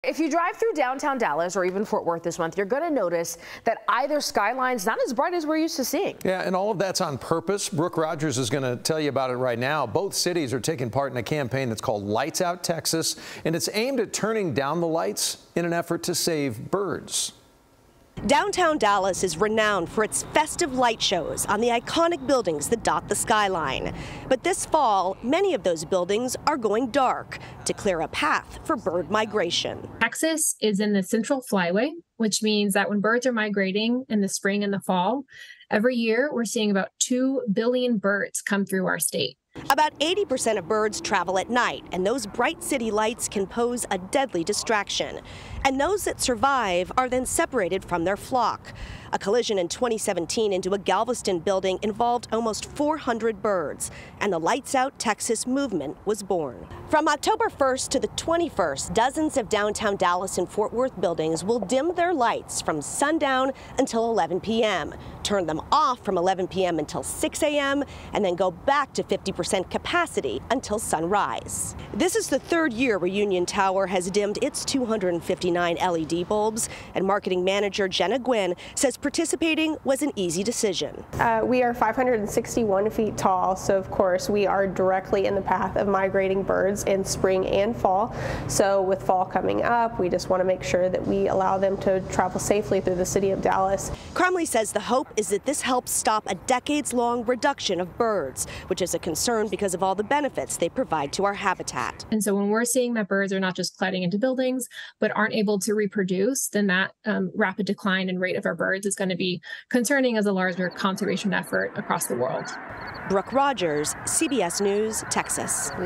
"Lights Out, Texas" campaign aims to dim city skylines for migrating birds
About 80% of birds travel at night, and those bright city lights can pose a deadly distraction.